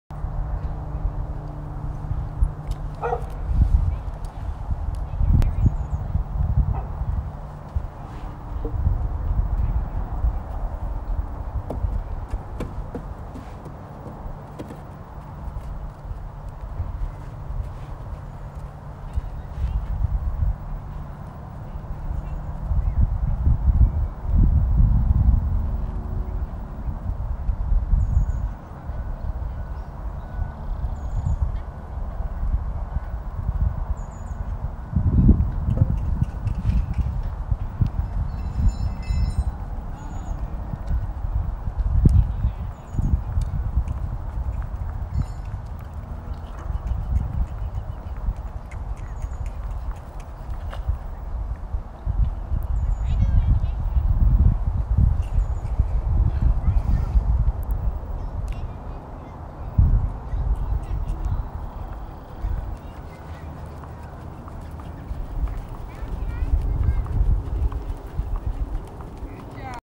Patternpalooza Class 41 - #286 Sidney & Harley
March 2021

Class 41 - Tadpole Trail WT/WJ
Exhibitor #286
Rider: Sidney
Horse: Harley